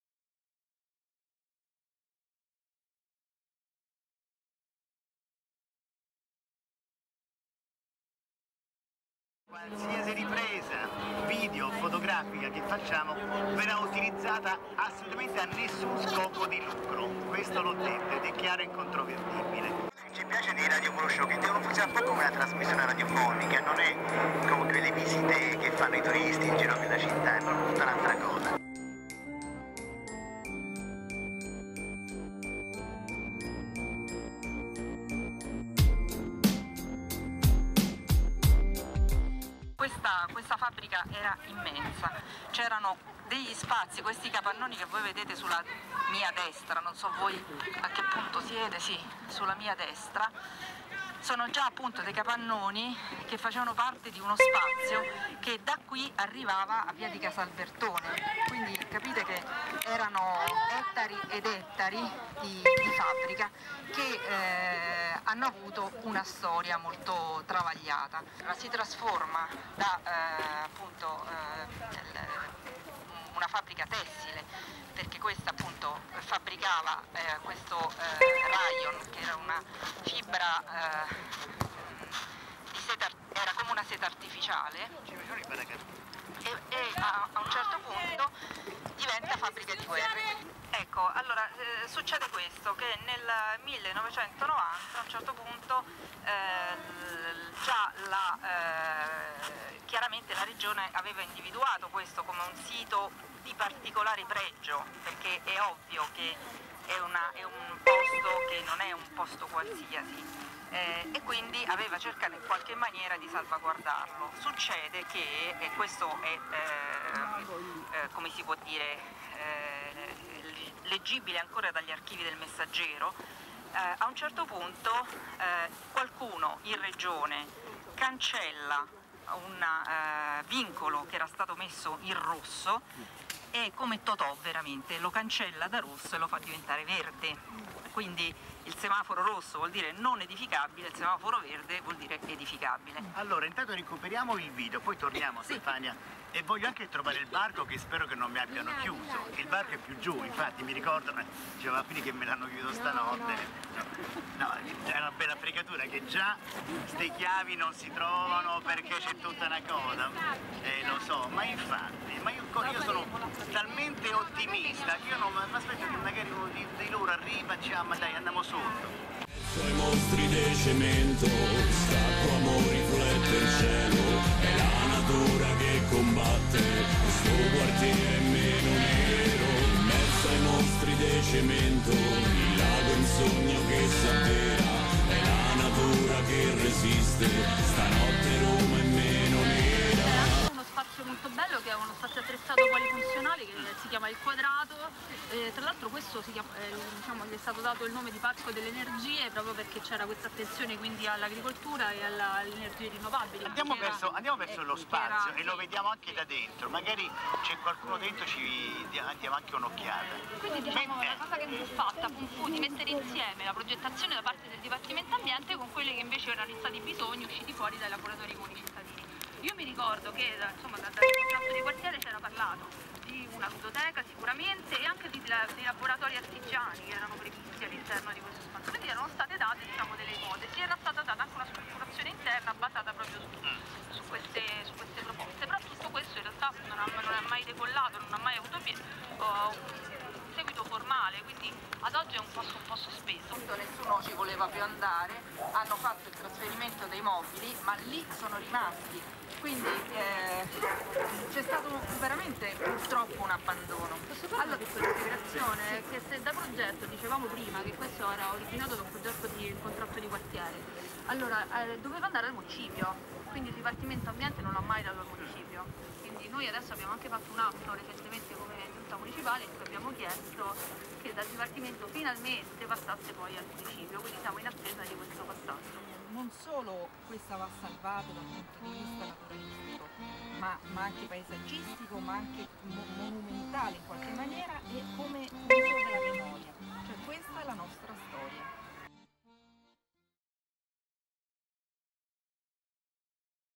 Radio- Walkshow "ll Lago di tutti"
Il video report della radio walkshow sul lago dell' ExSnia per il progetto Piedi per Terra e Testa nel Cloud.
Riprese e montaggio: Paolo Pitotti